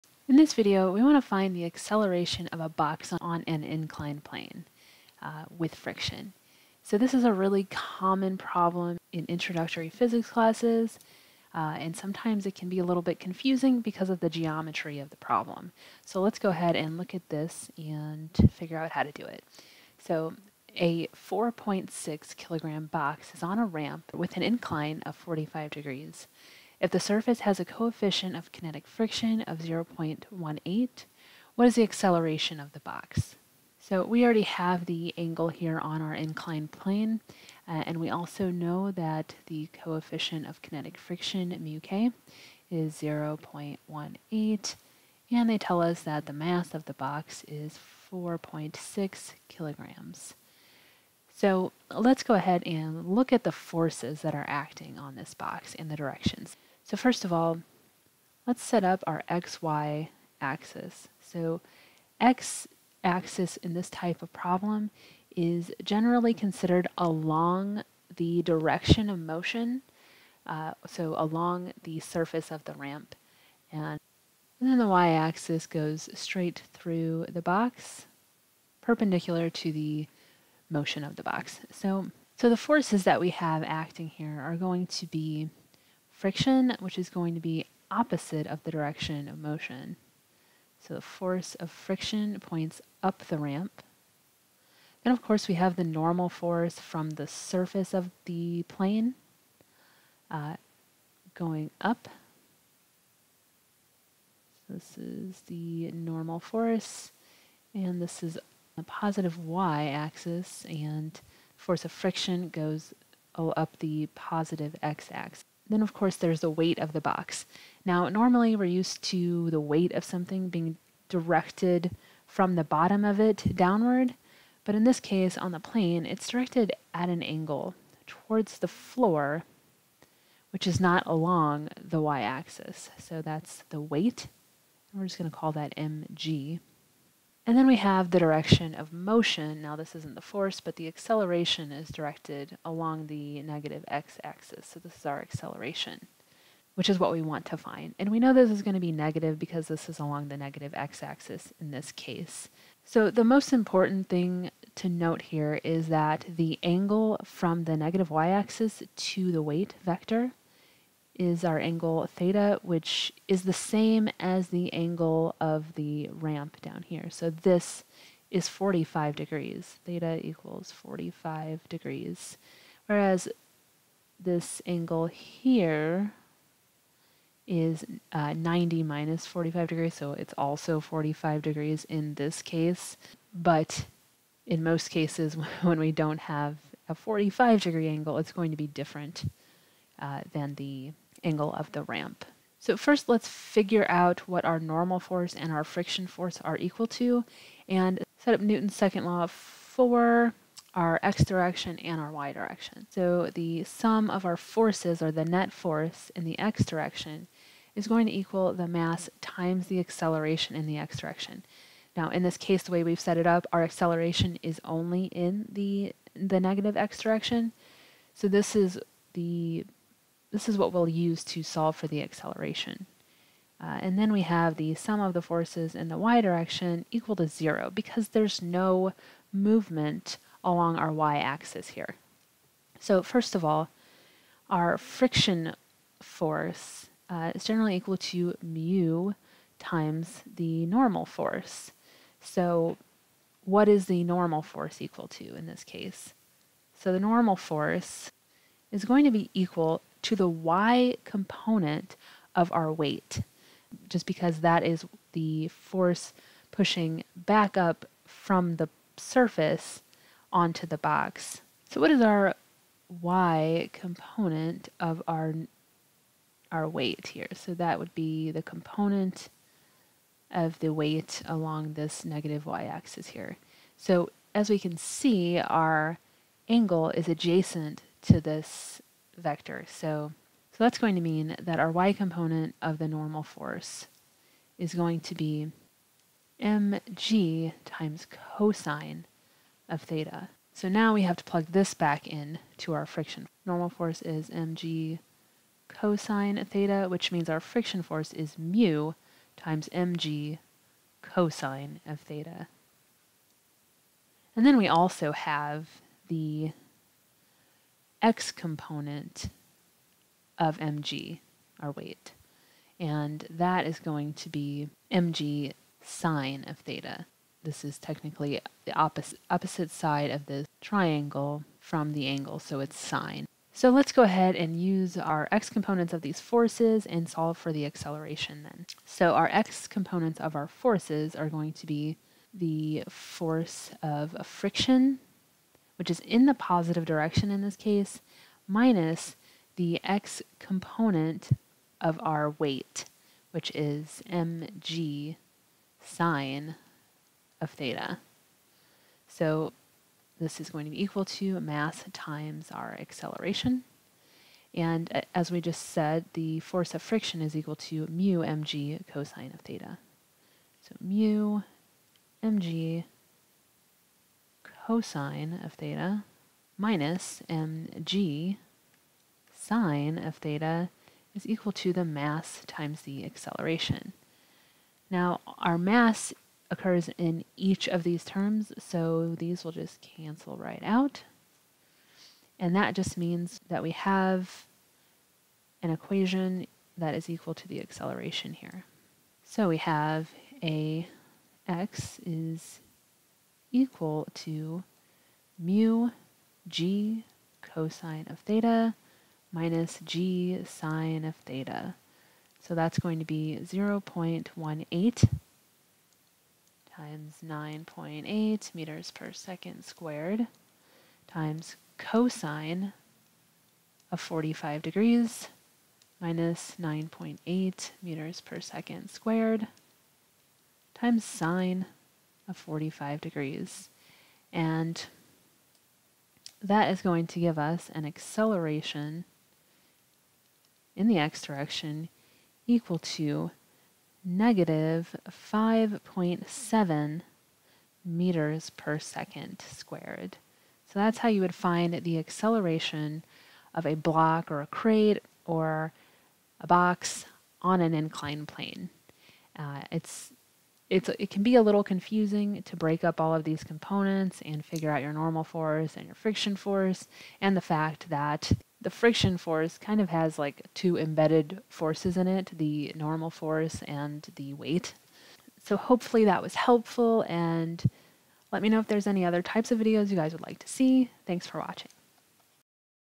Force and Acceleration: Box on an inclined plane (with friction)
How to find the acceleration of a box sliding down a ramp with friction using forces.